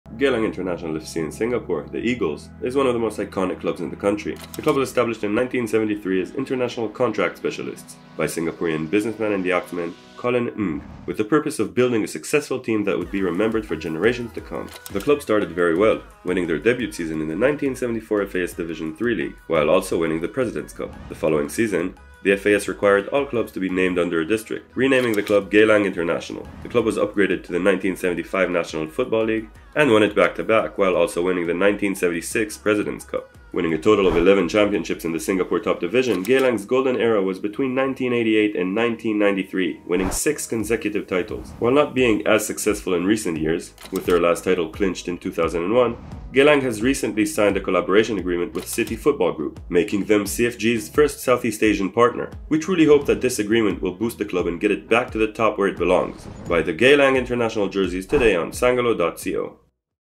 Get to Know the Football Club: Geylang International FC from Singapore
Geylang International FC is a legendary club from Singapore! You can't buy history and Geylang have some history!